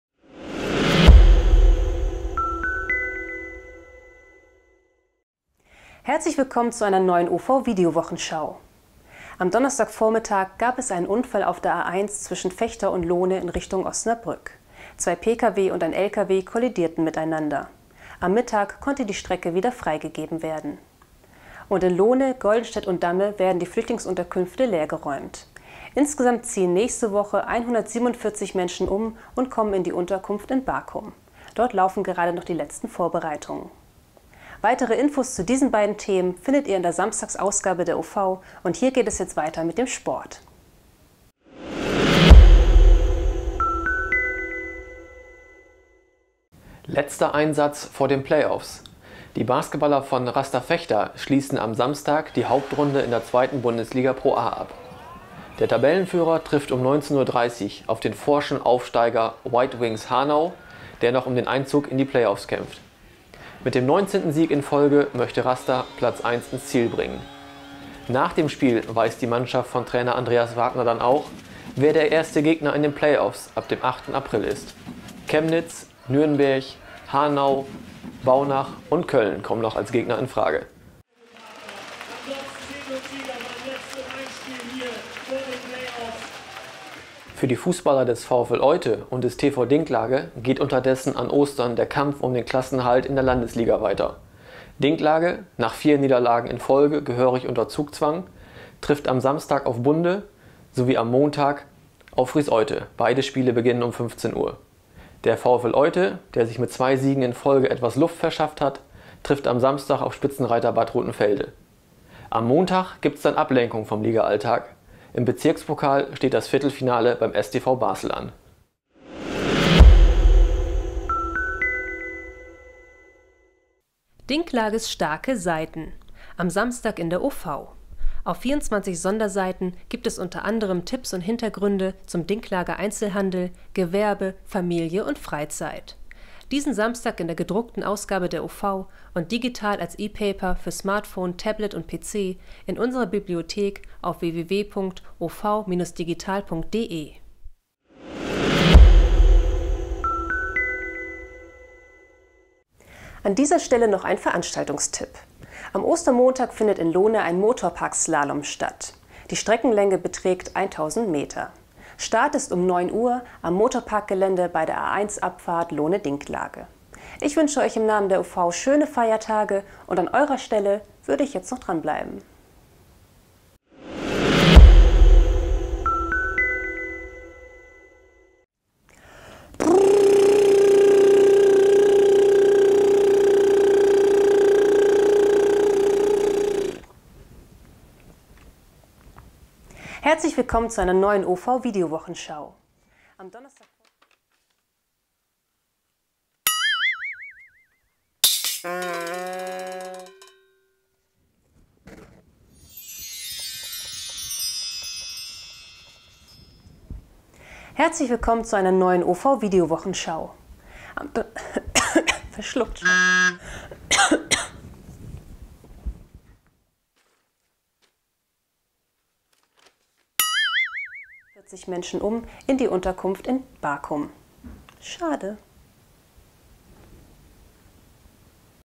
OV-Video-Wochenschau 24.03.16
In dieser Wochenschau gibt es Meldungen des Tages, einen Überblick auf das Sportwochenende von Rasta Vechta, dem VfL Oythe und TV Dinklage. Außerdem wollen wir euch zu den Feiertagen noch ein paar Aufnahmen außer der Reihe zeigen... Viel Spaß!